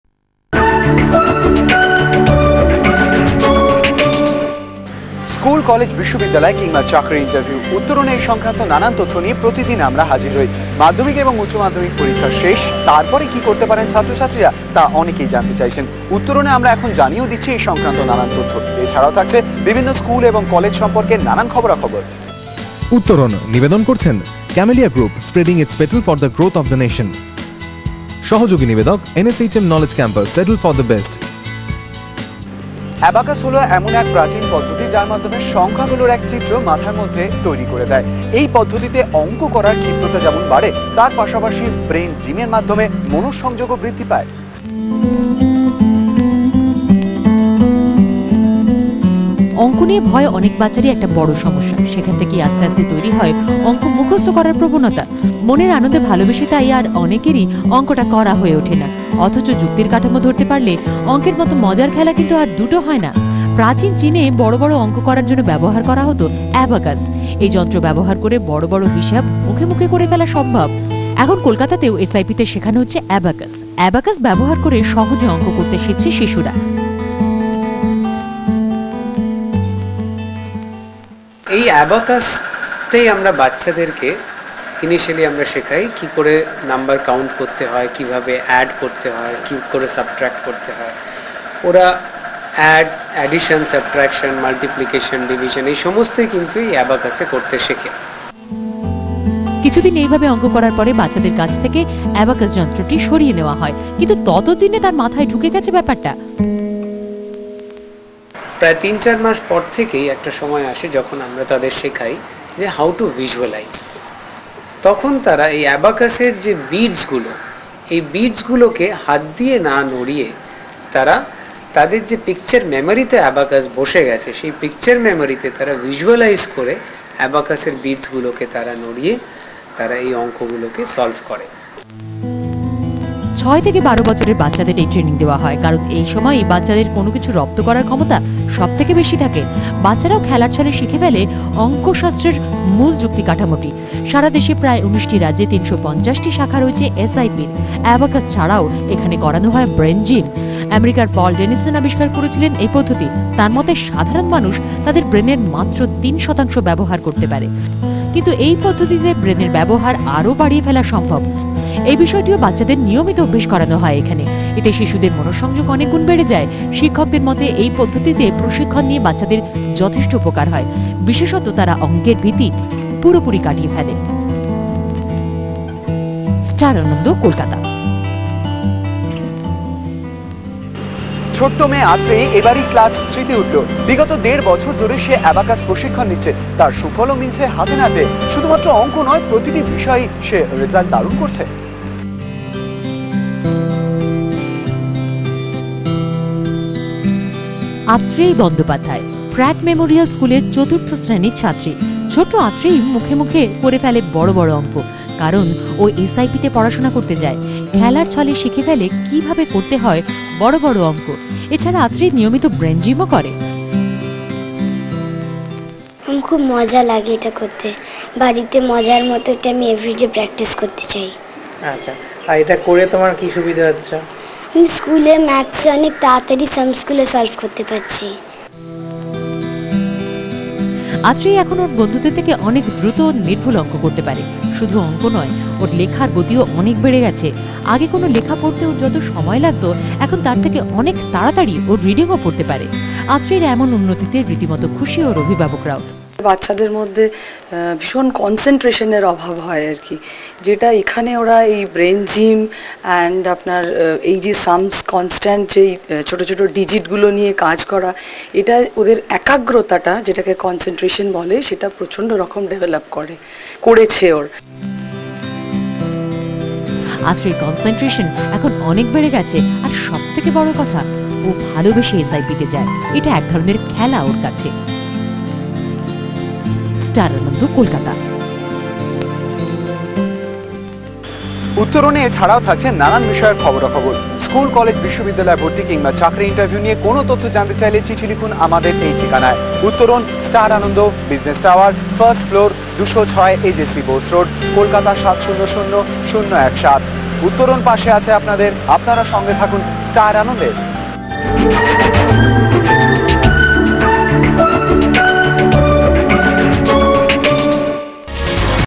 SIP Abacus & Brain Gym - Star Ananda Tv
This is a news coverage on Abacus & Brain Gym programme of SIP ABACUS & BRAIN GYM for school children by the no.1 Bengali News Channel & i.e. STAR ANANDA